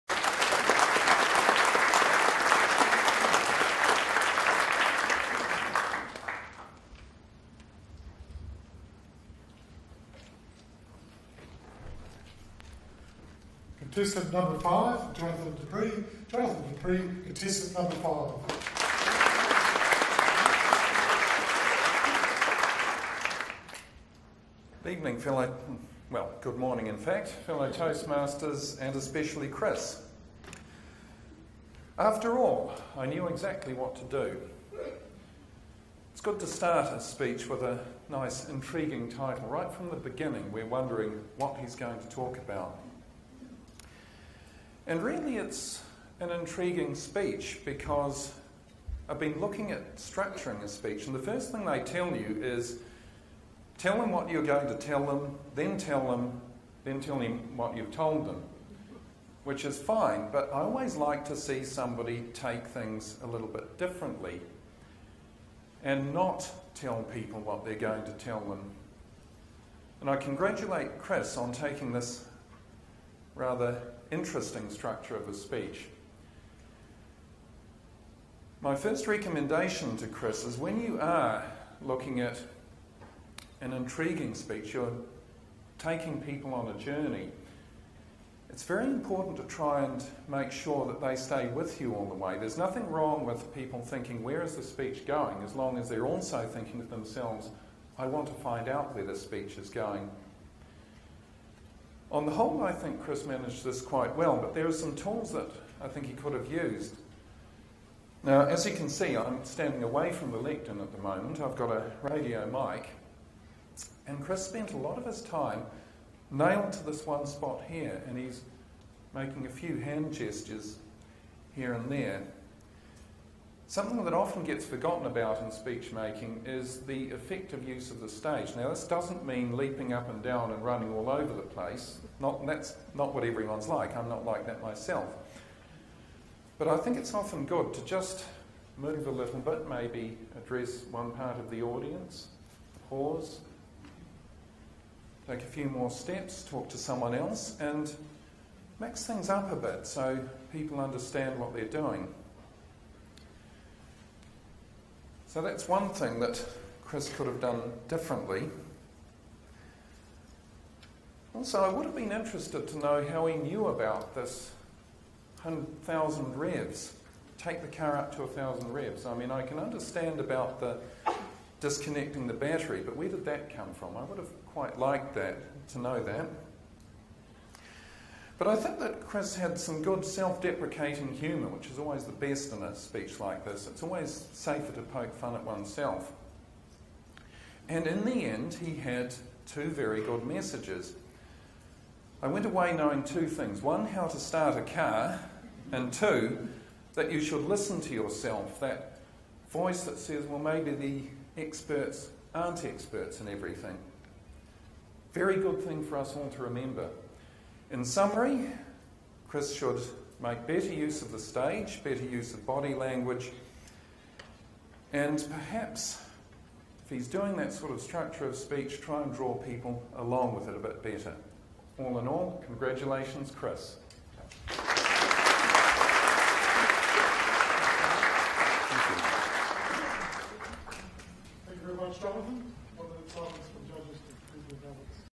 Jonathan Du Pree - Evaluation Contest 2016 Toastmasters NZ
Jonathan Du Pree competes in the Evaluation Contest at the Toastmasters District 72 Convention in May 2016 in Rotorua, New Zealand.